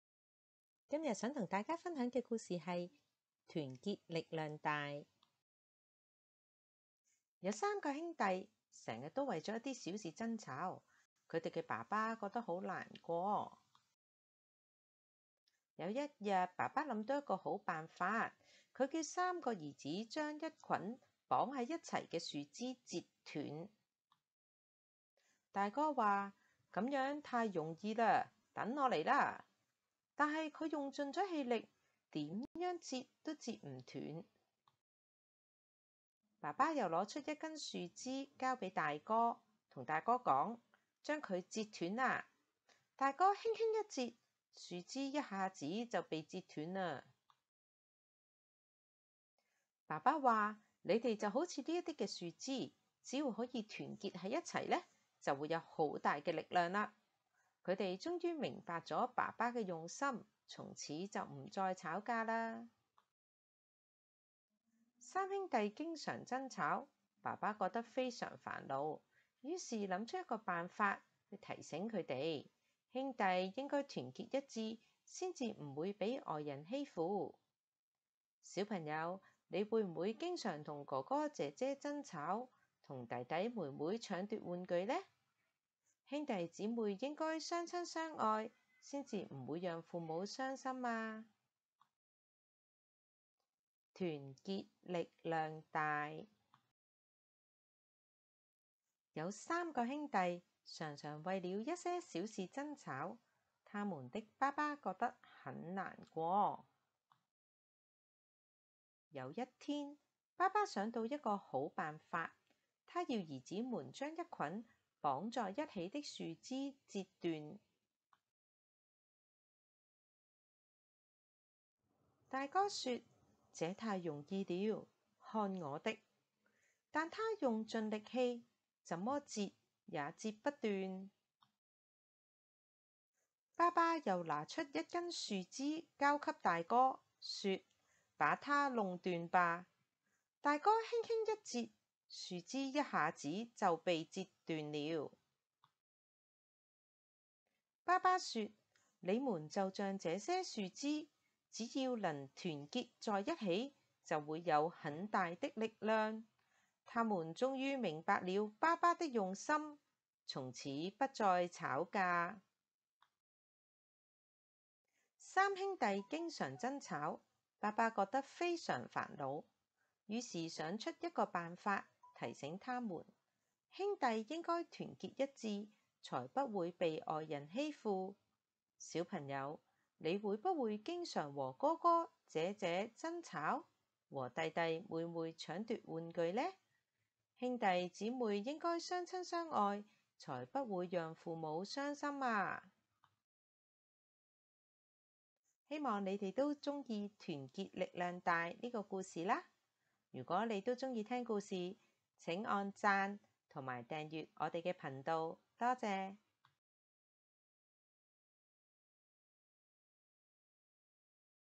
團結力量大 | 廣東話故事 |粵語故事
心靈成長系列︰團結齊心
兄弟姊妹經常爭吵，怎麼辦呢？
其實兄弟姊妹應該相親相愛，團結一致，才能發揮最大的力量！